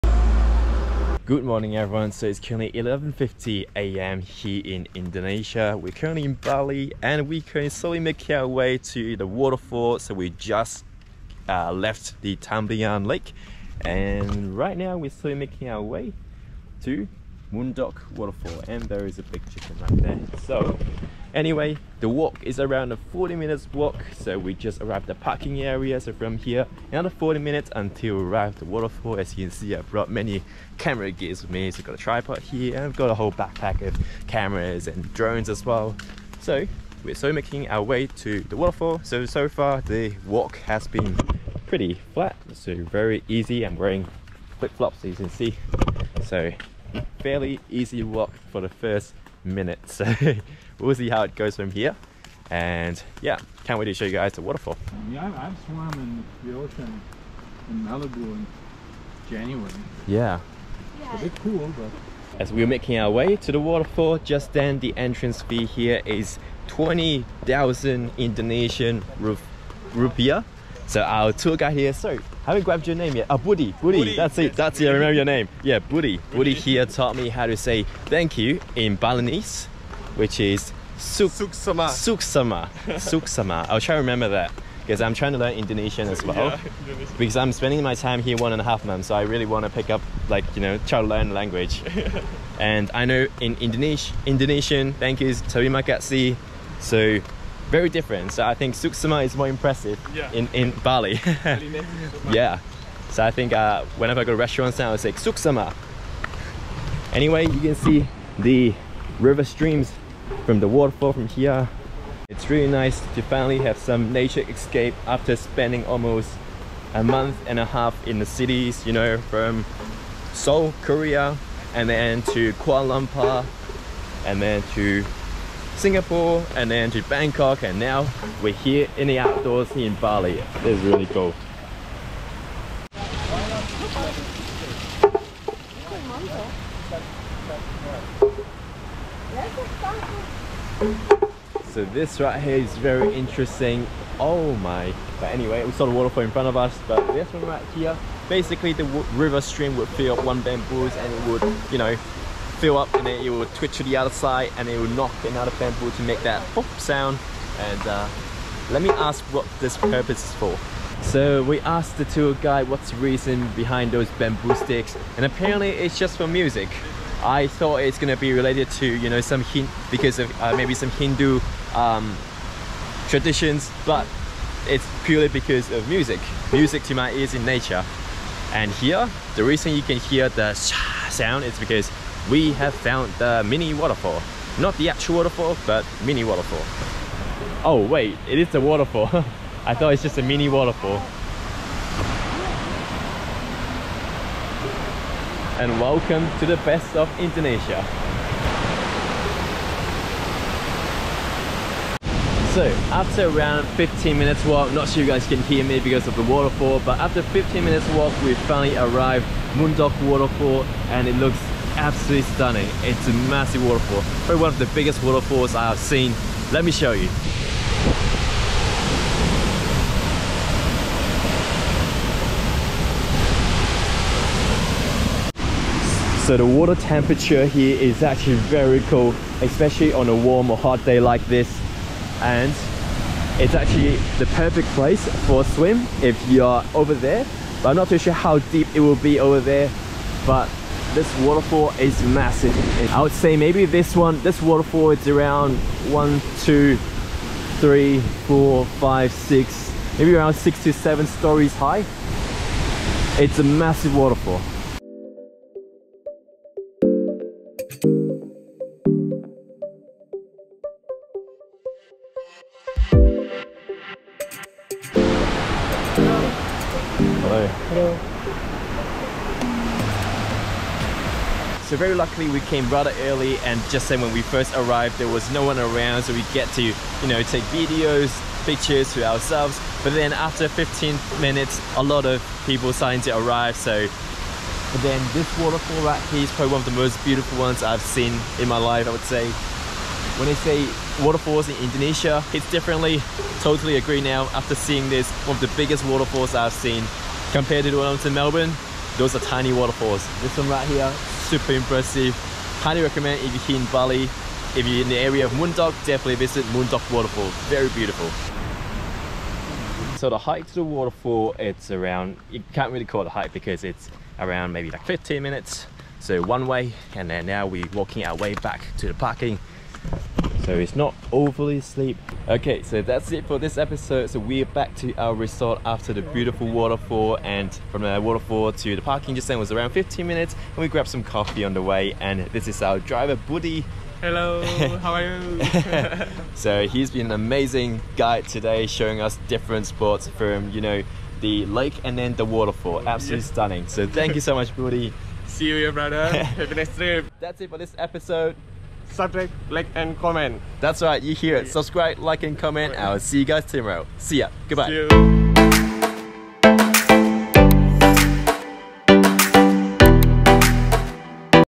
Munduk Waterfall - This Waterfall is AMAZING. Best Attraction in North Bali
In this episode, I visited Munduk Waterfall, a tall waterfall situated in a northern area in Bali. The waterfall was spectacular and it felt like it was around 6- storey high and it had the waterfall to ourselves for a brief 10-15 minutes. I finally understand when people said waterfall in Bali is one of a kind and this is only the beginning of our adventure in Indonesia.

Google Map Address: Munduk Waterfall Parking (15 minutes walk to the waterfall)

RESOURCES!